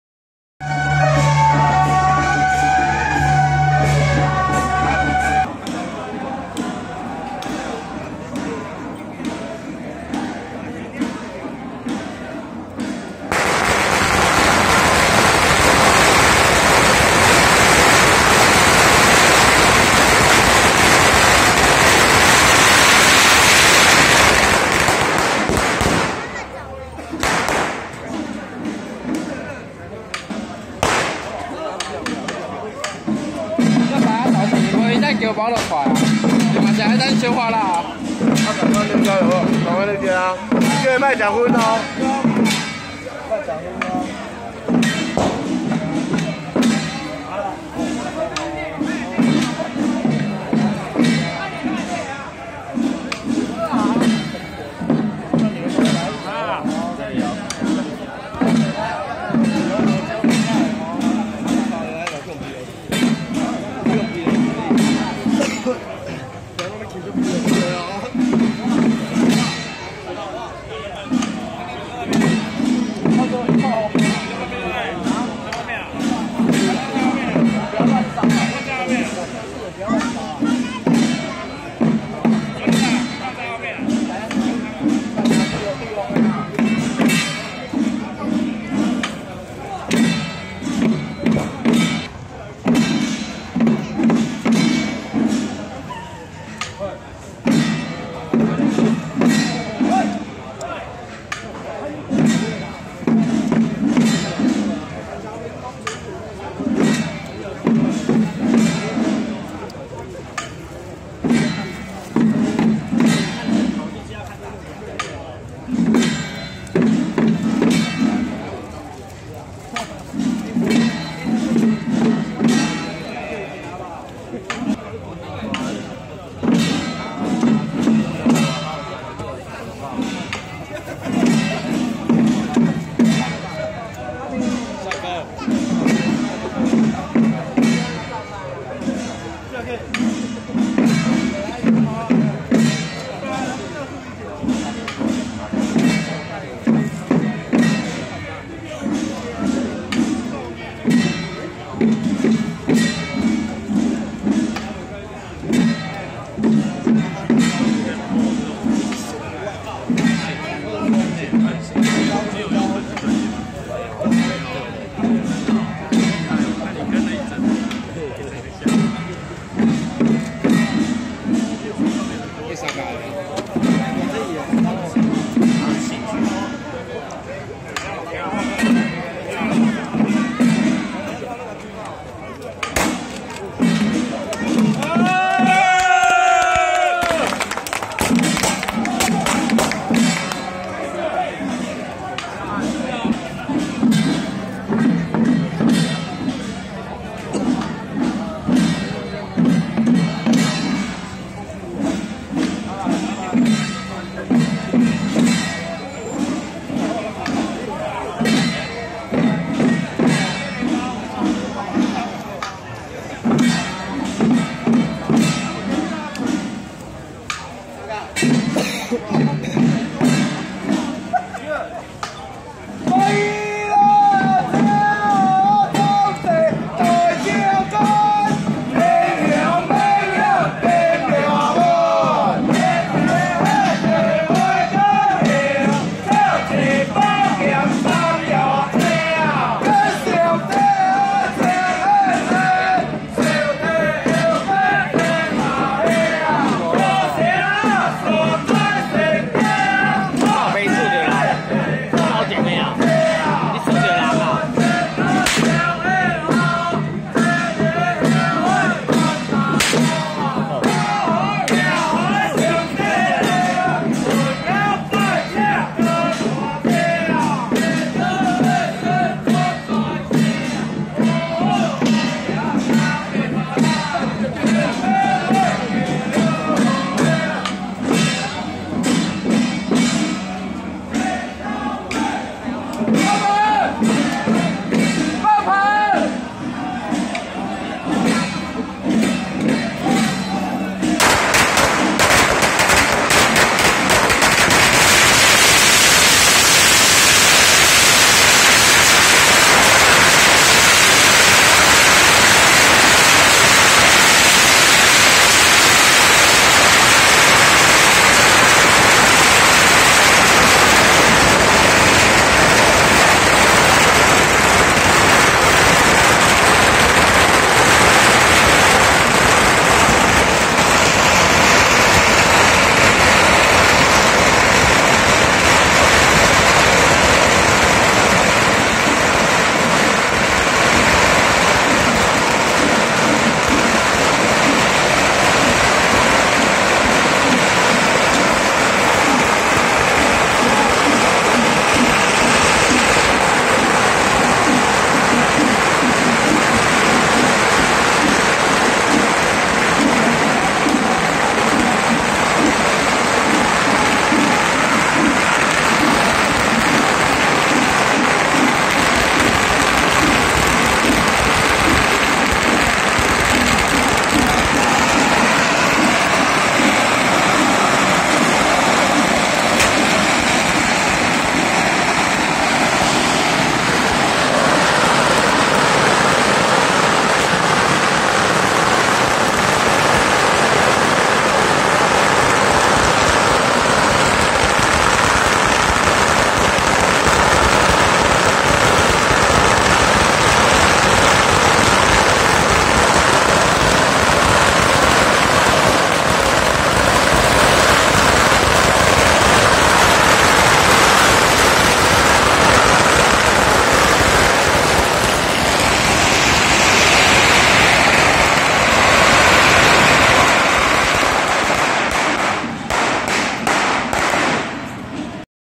106.12/24板橋高家吳府千歲往臺南南廠保安宮鑑醮回駕遶境【板橋高家-涼傘＆吳府千歲】
板橋高家-涼傘＆吳府千歲